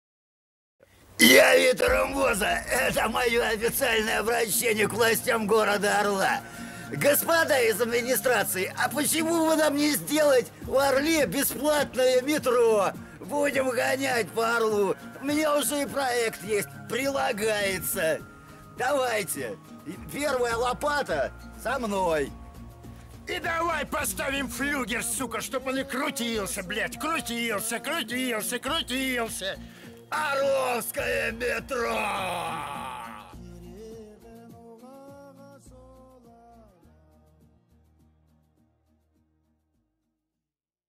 Орловское Метро Легендарный проект!!!
Господа! Разработан генеральный план строительства в Орле метрополитена.
Любому понятно, что метро Орлу просто необходимо! Необходимо, чтобы сократить расстояния, решить проблему пробок и не путаться в номерах маршрутов. Чтобы заставить нас гордиться нашим городом и областью, стать настоящими патриотами нашей малой Родины! Я уж не говорю об экономических эффектах данного начинания. Сколько инвестиций оно принесет Орлу!